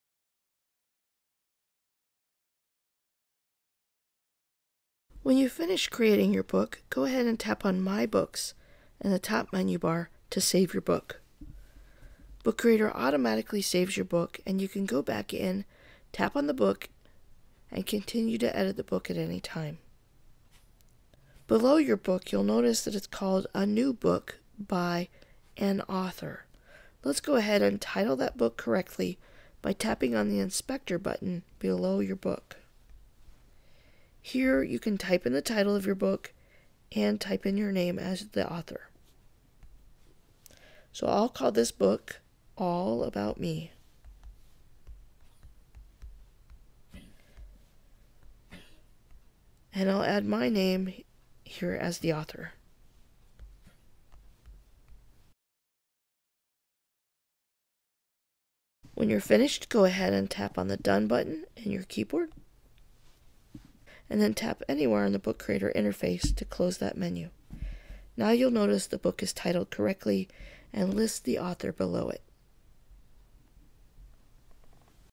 Book Creator - Changing The Book Title And Author
How to change the title of the book and the author in the Book Creator app on the iPad.